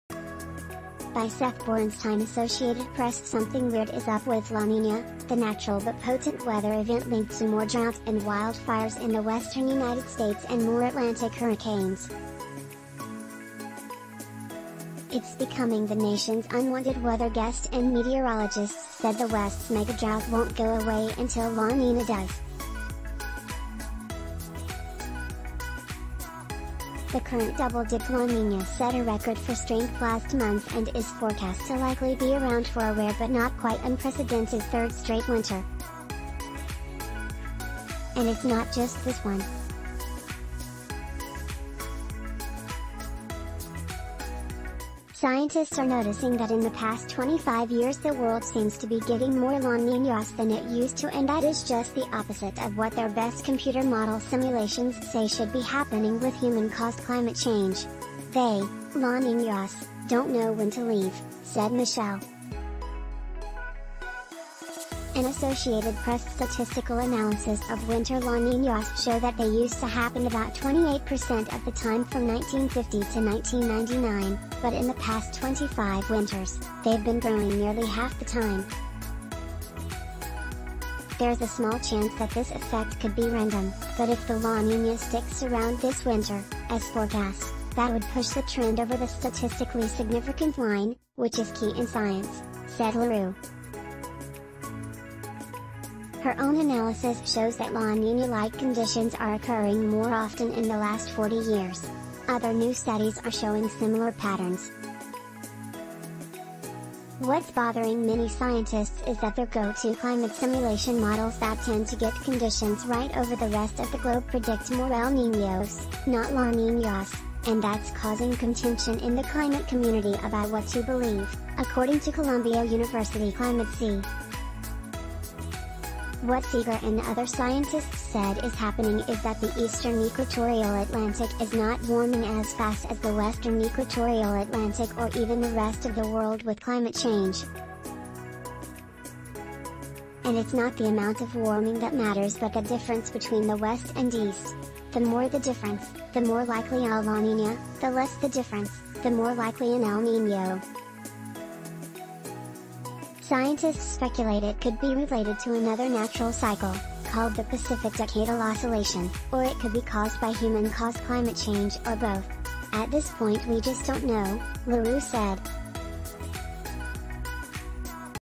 Weather’s unwanted guest Nasty La Niña keeps popping up
By SETH BORENSTEIN | Associated PressSomething weird is up with La Niña, the natural but potent weather event linked to more drought and wildfires in the western United States and more Atlantic hurricanes. It’s becoming the nation’s unwanted weather guest and meteorologists said the West’s megadrought won’t go away until La Nina does. The current double-dip La Niña set a record for strength last month and is forecast to likely be around for a rare but not quite unprecedented third straight winter. And it’s not just this one. Scientists are noticing that in the past 25 years the world seems to be getting more La Niñas than it used to and that is just the opposite of what their best computer model simulations say should be happening with human-caused climate change.“They (La Niñas) don’t know when to leave, ” said Michelle L’Heureux, head of the National Oceanic and Atmospheric Administration forecast office for La Nina and its more famous flip side, El Niño. An Associated Press statistical analysis of winter La Niñas show that they used to happen about 28% of the time from 1950 to 1999, but in the past 25 winters, they’ve been brewing nearly half the time. There’s a small chance that this effect could be random, but if the La Niña sticks around this winter, as forecast, that would push the trend over the statistically significant line, which is key in science, said L’Heureux. Her own analysis shows that La Niña-like conditions are occurring more often in the last 40 years. Other new studies are showing similar patterns. What’s bothering many scientists is that their go-to climate simulation models that tend to get conditions right over the rest of the globe predict more El Niños, not La Niñas, and that’s causing contention in the climate community about what to believe, according to Columbia University climate scientist Richard Seager and MIT hurricane scientist Kerry Emanuel. What Seager and other scientists said is happening is that the eastern equatorial Atlantic is not warming as fast as the western equatorial Atlantic or even the rest of the world with climate change. And it’s not the amount of warming that matters but the difference between the west and east. The more the difference, the more likely a La Niña, the less the difference, the more likely an El Niño. Scientists speculate it could be related to another natural cycle, called the Pacific Decadal Oscillation, or it could be caused by human-caused climate change or both.“At this point we just don’t know, ” L’Heureux said. “Scientists are watching and I know, are actively studying. But it’s really important because of regional conditions. We need to get this right. ”La Niña is a natural and cyclical cooling of parts of the equatorial Pacific that changes weather patterns worldwide, as opposed to El Niño’s warming.